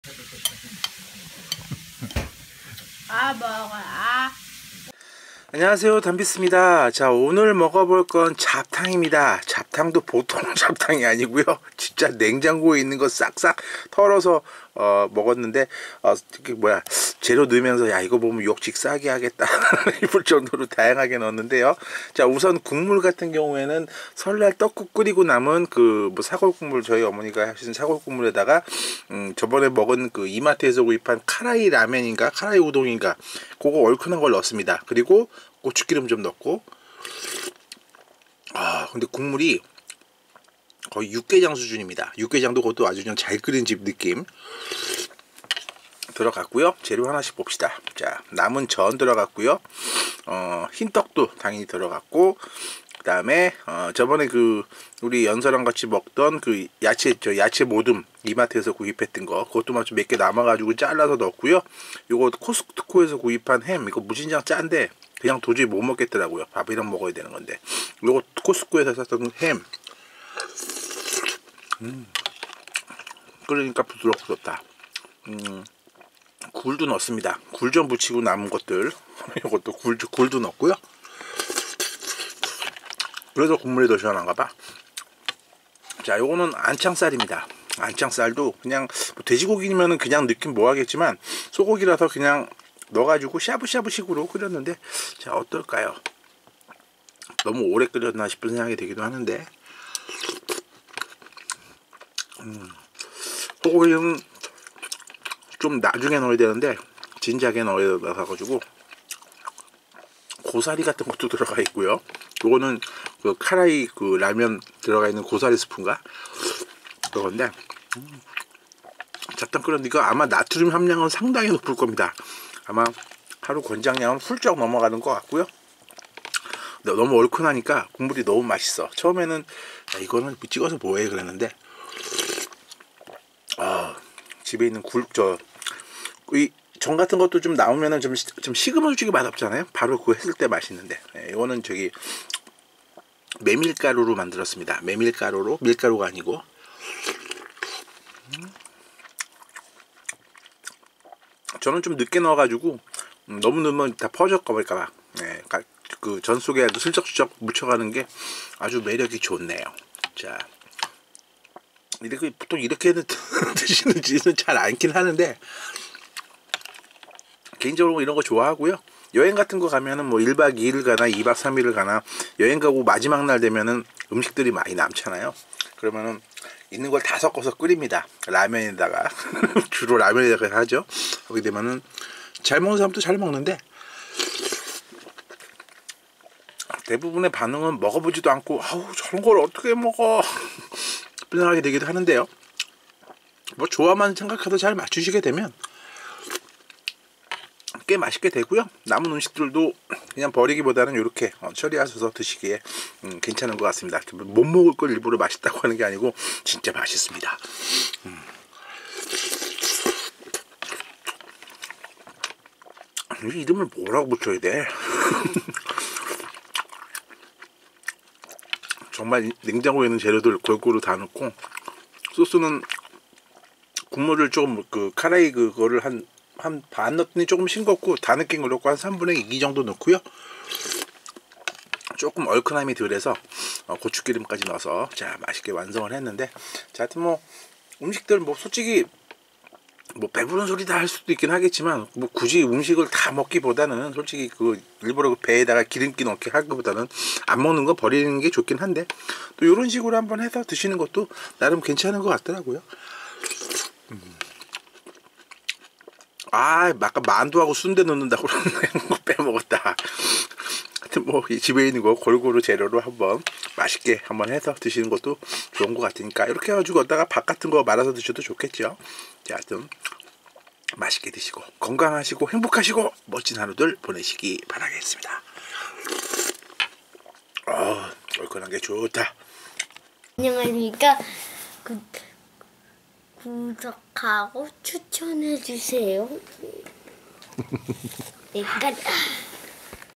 잡탕, 명절 남은 음식들을 넣고 끓인 탕 국물 요리 시식기
설날을 쉬고나서 남은 음식들을 넣어서 잡탕을 먹어봤는데, 맛있네요...^^